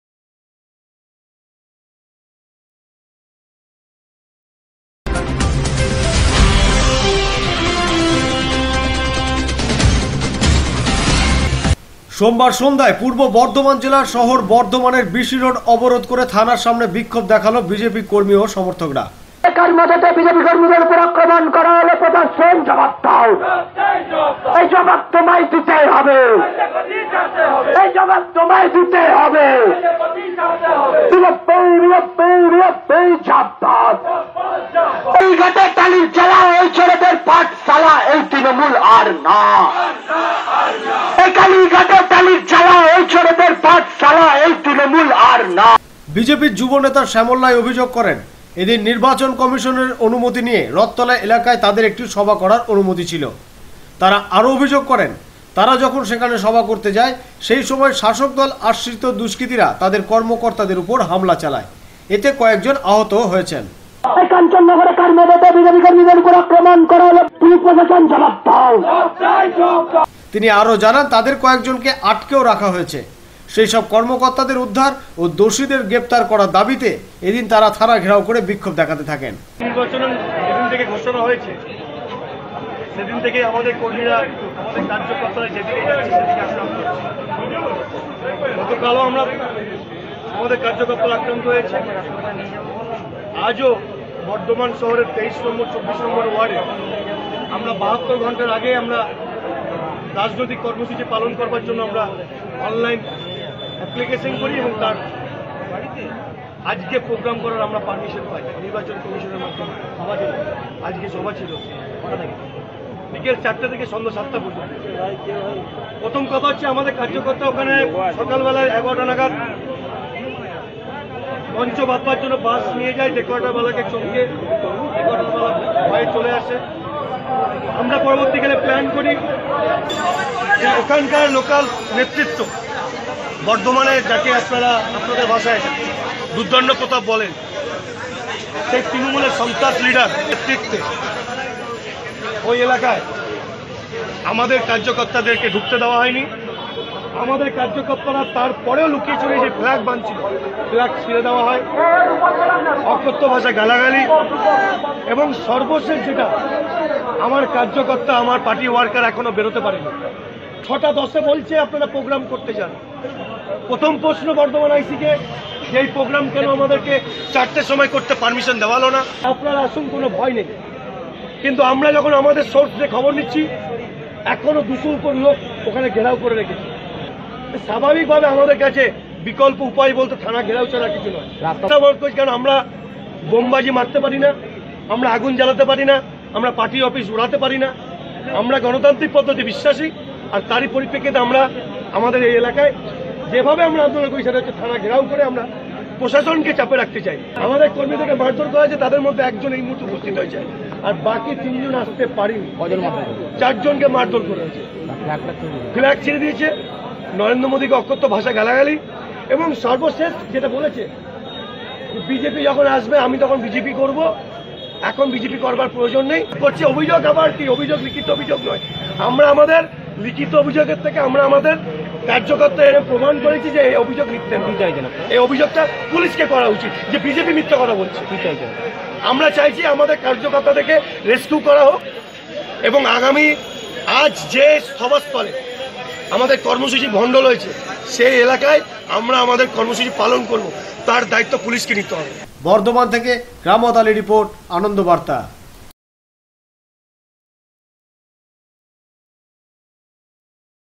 BC ROAD ABORODH//বর্ধমান থানার সামনে বিসি রোড অবরোধ বিজেপি র
বিজেপির জনসভায় দুস্কৃতির হামলার প্রতিবাদে বিসি রোড অবরোধ করে
বর্ধমান থানার সামনে ঘেরাও করলো বিজেপি কর্মী সমর্থকরা।